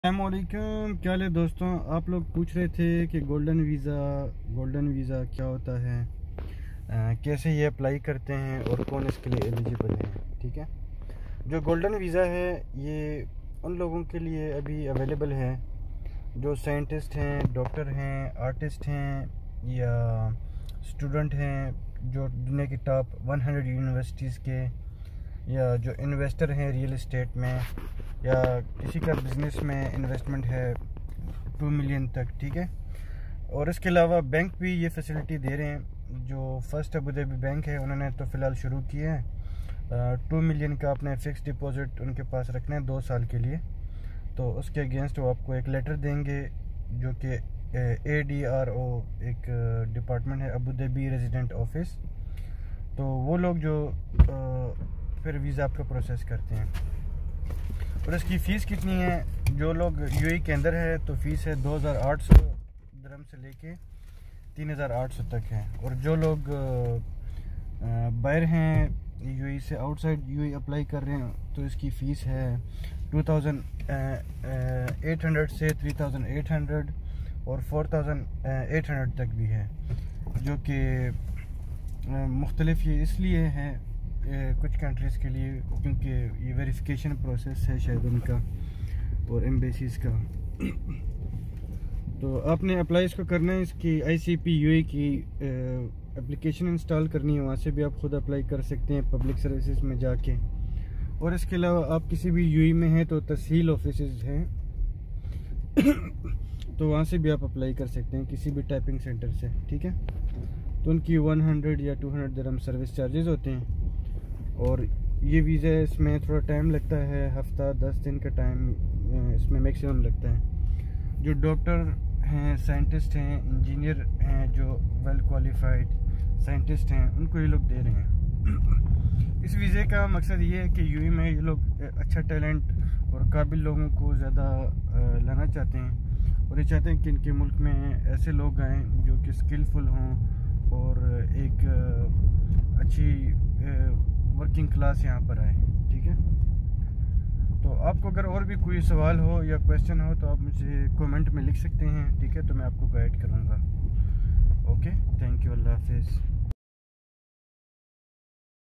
how to apply uae golden visa details.
دبئی میں گولڈن ویزہ کی مکمل معلومات۔اگر کوئی سوال ہو تو کمنٹ میں پوچھ سکتے ہیں۔
how to apply uae golden visa?
how much is fees ?2800 to 3800 aed